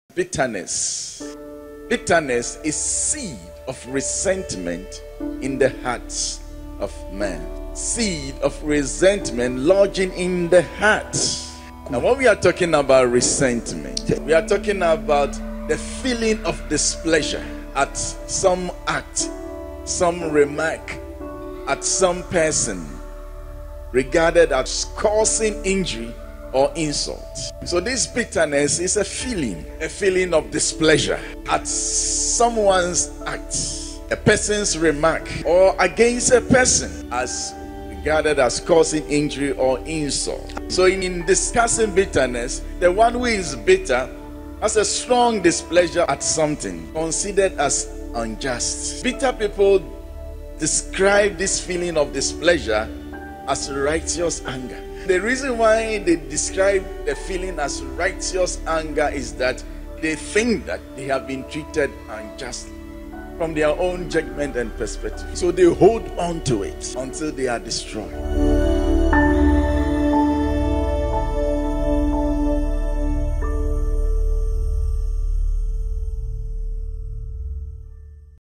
Bitterness Can Destroy You | Apostle Eric Nyamekye
“Bitterness is a feeling; a feeling of displeasure at someone’s act or against a person. Bitterness can destroy you.” – Apostle Eric Nyamekye